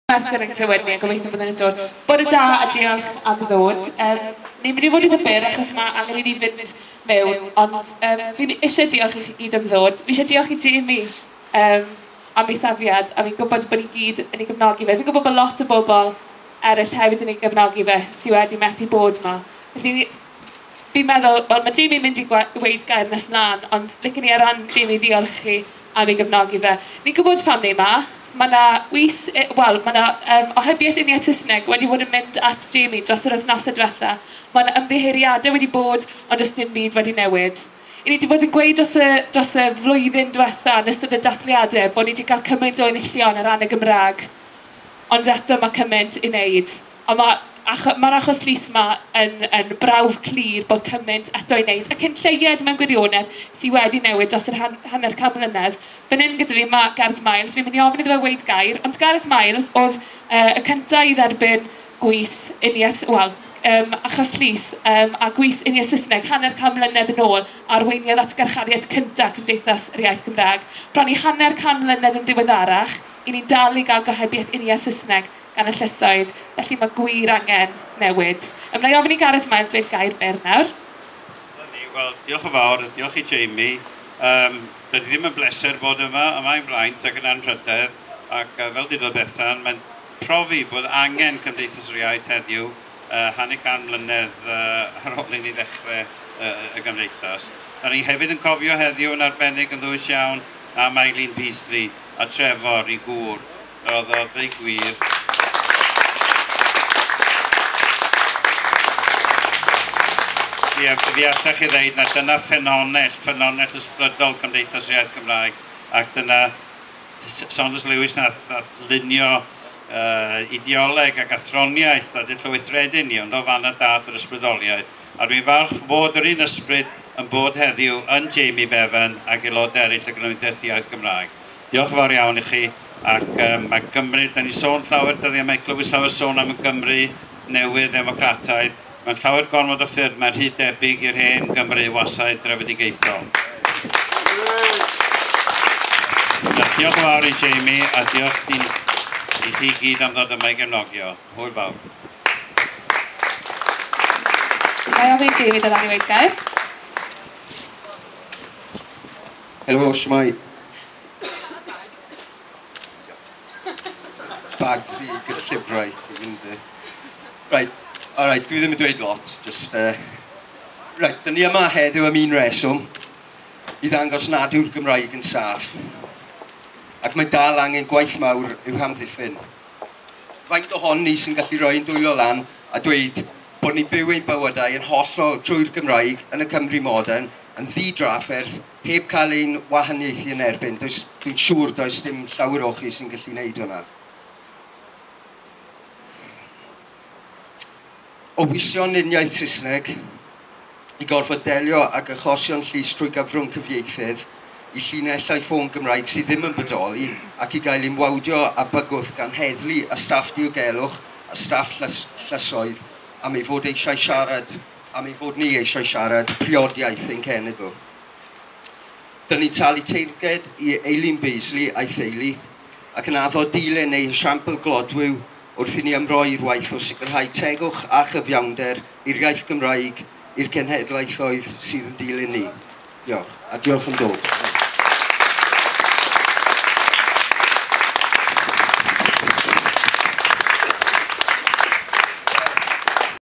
Areithiau cyn achos llys Jamie Bevan - gyda Gareth Miles a Bethan Williams
Jamie Bevan - Carchar dros yr iaith
13eg mis Awst 2012

Siaradwyr
Bethan Williams
Gareth Miles
Jamie Bevan